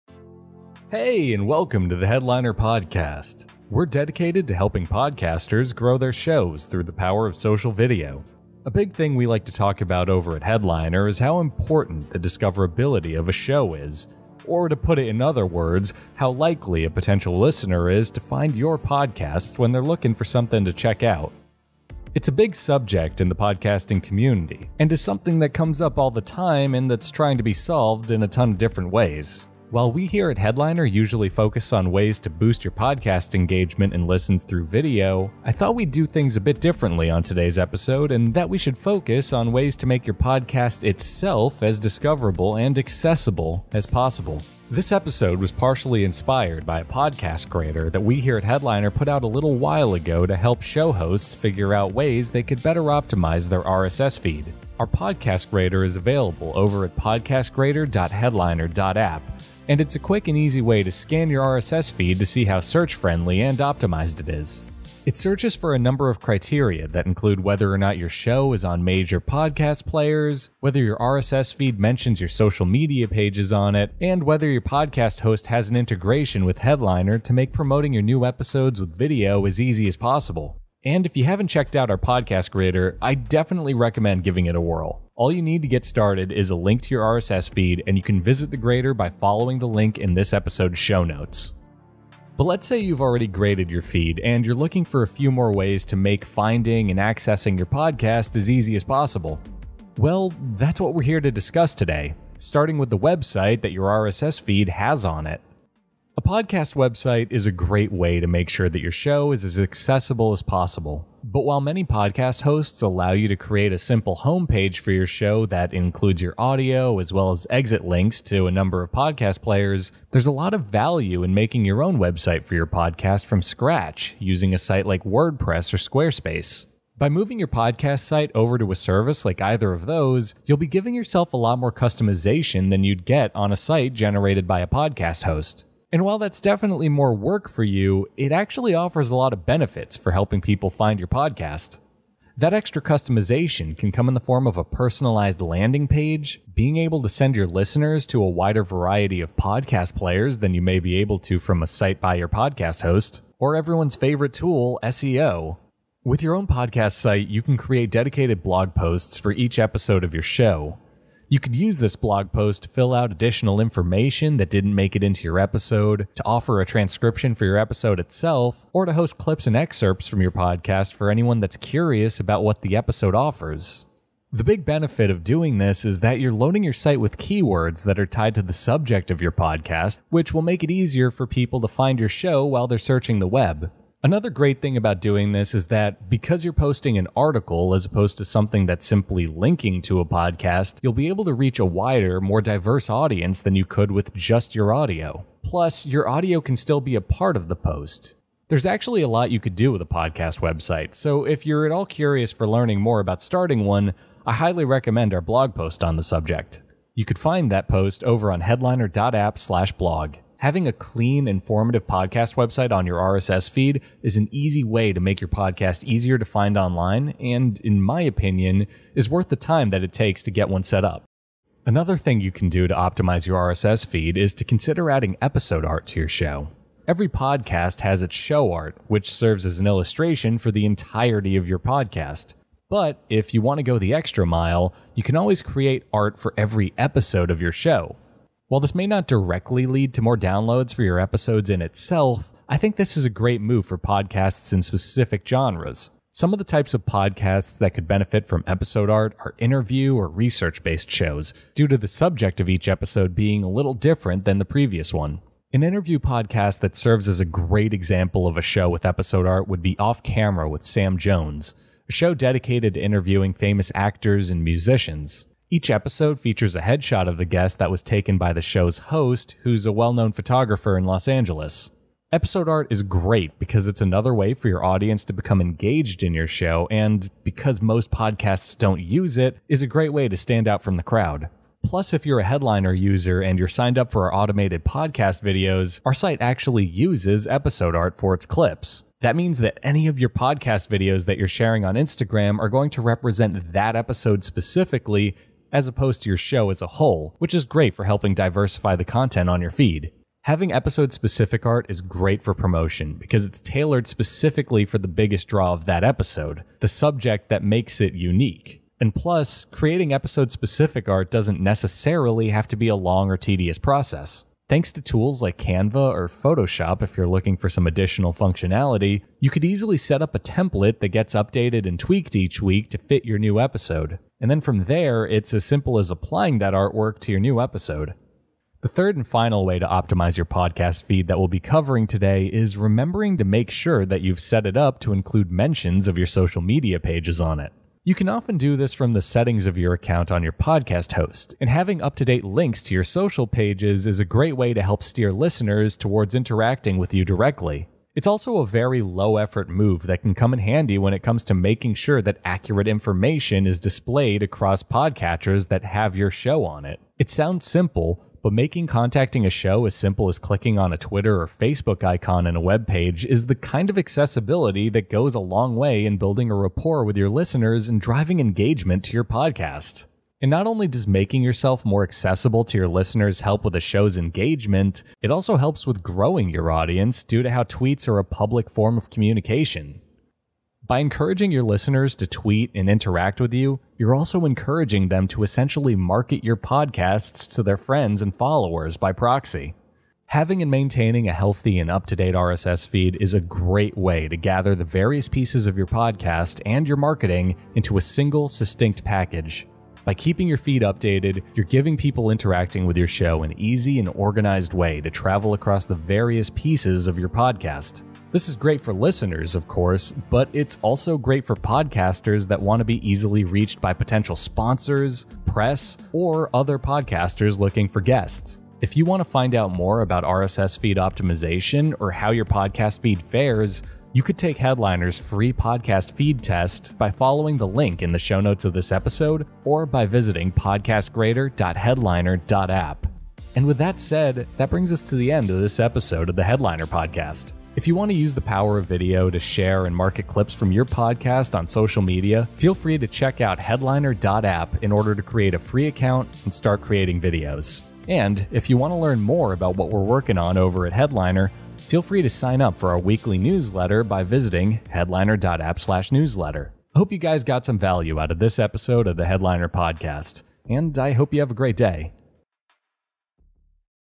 Episode 009: Optimizing Your Podcast Feed
On this week's Headliner Podcast, Nick from the Headliner team goes over a few ways to optimize & get the most out of your RSS feed and how it can help you grow your show.

This episode was uploaded thanks to Make by Headliner's auto-posting YouTube integration. Interested in giving it a spin for yourself?